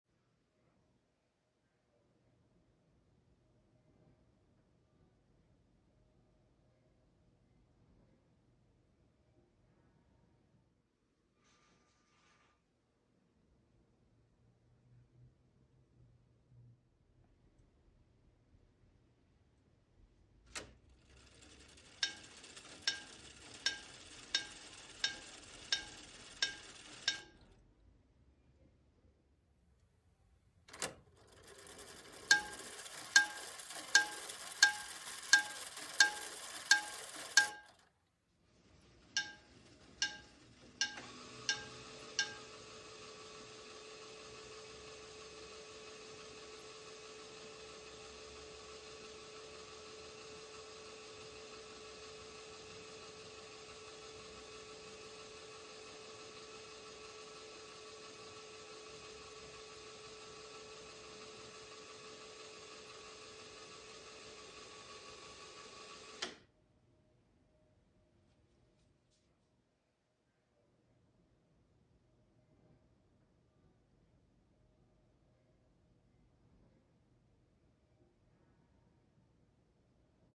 LEGO Grandfather Clock 2 December 25, 2022
From my Original Grandfather clock I added a chime striker this weekend.
Additional complications now include:
Weights for both clock and chimer functions, each with auto-winders.
Coaxial Day / Night and Lunar Phase indicator.
The H-Loop chain tensioners are now incorporated into the clock foot instead of hanging free.